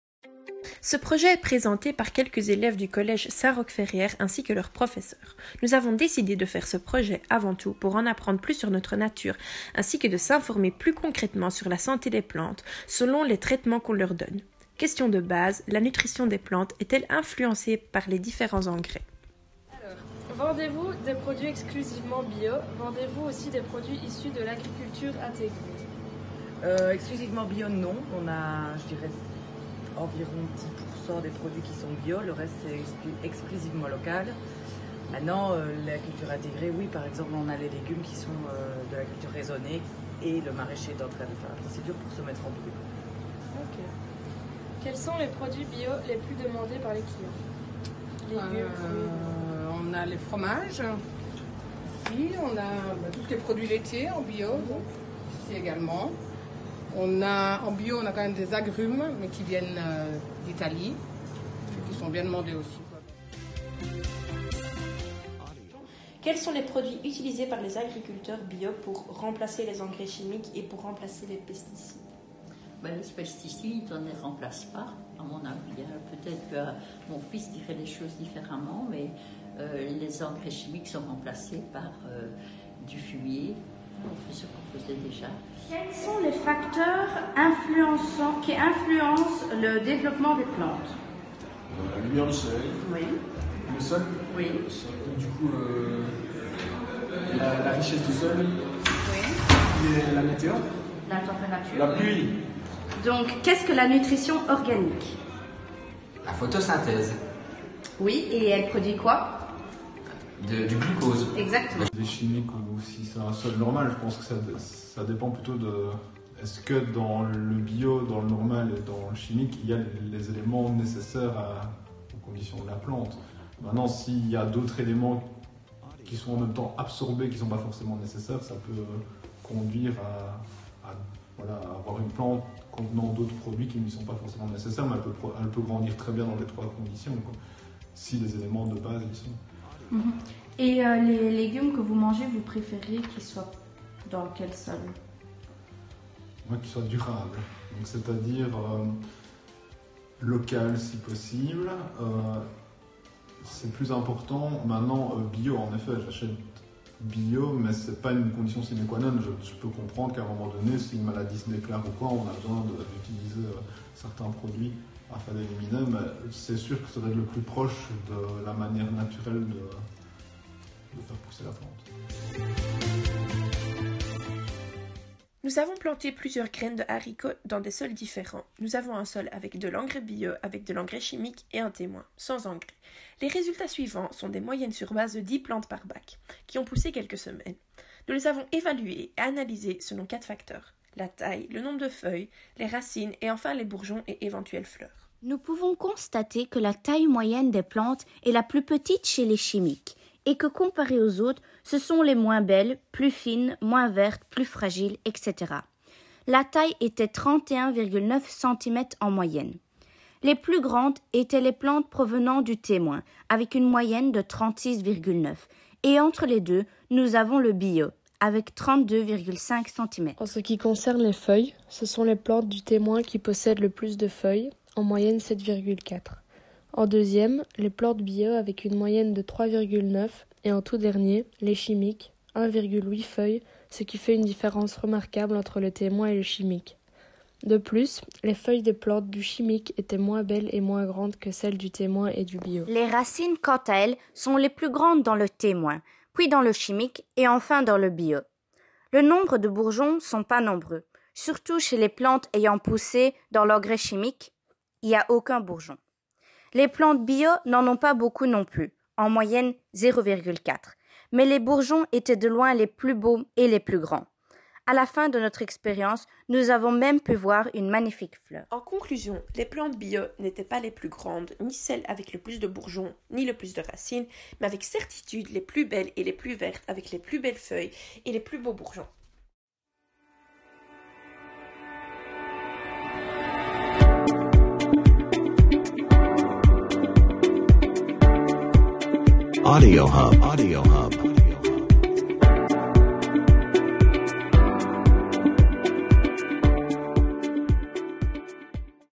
La santé des plantes - Collège Saint-Roch Ferrières
Une vidéo réalisée dans le cadre de "Science Video challenge 2020 sur la santé des végétaux" par trois élèves de 4e B (Mira, Anouk et Lisa) sous la houlette de M. Hamad Karous. Bravo à elles !